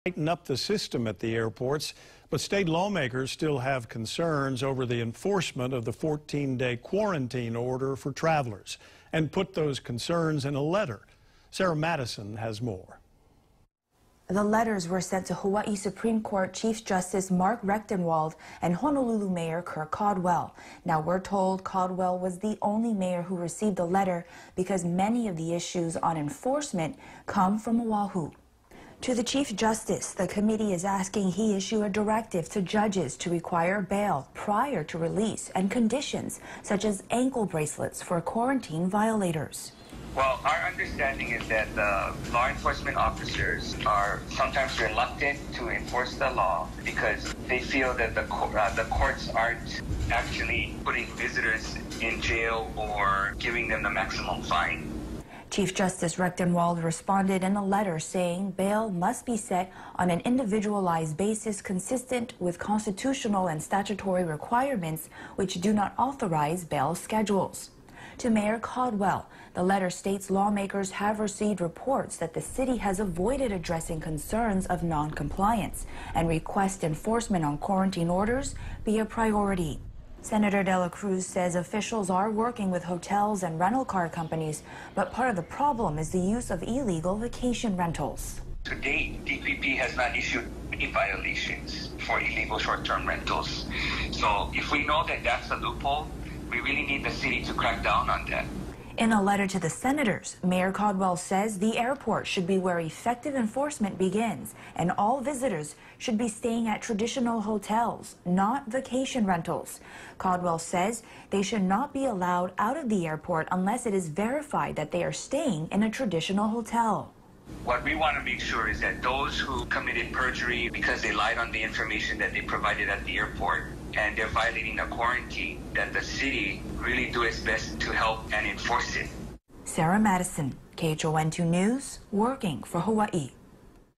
Senators express concern over enforcement of 14-day quarantine order for travelers
Officials are trying to tighten up the system at the airports. But state lawmakers still have concerns over the enforcement of the 14-day quarantine order for travelers and put those concerns in a letter.